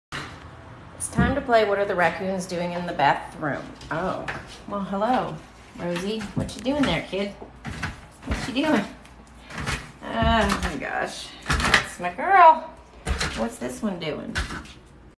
Rosy the Toilet Paper Crackcoon || ViralHog
Occurred on September 27, 2020 / USA

"I am rehabbing her for release and she comes and goes through the doggie door as she pleases. She also uses the bathroom as she pleases when I forget to shut the door. She makes me laugh every day. I’ll miss her tons but raccoons are not pets, they are wild animals. But boy do they love like none other."